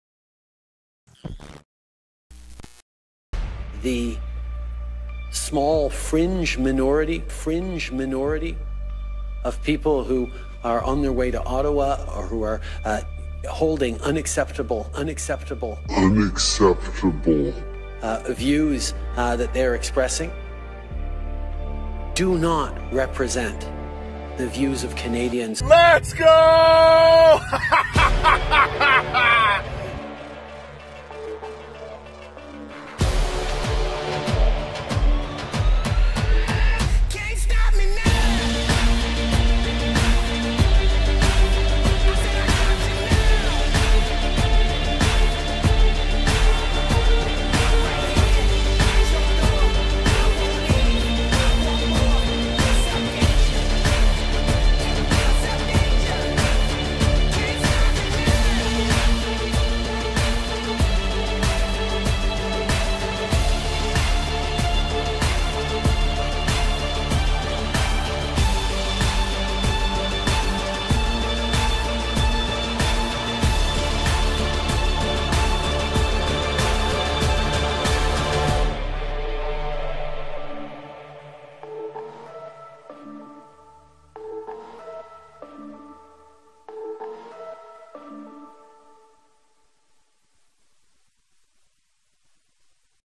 Canadian Truckers Strike
Where is Justin Trudeau? Canada CanadianTruckers CanadaHasFallen CanadaRebellion2022 CanadaConvoy JustinTrudeau My contribution to the Convoy. Vive la résistance! Americans, follow the example of the TrueNorthStrongAndFREE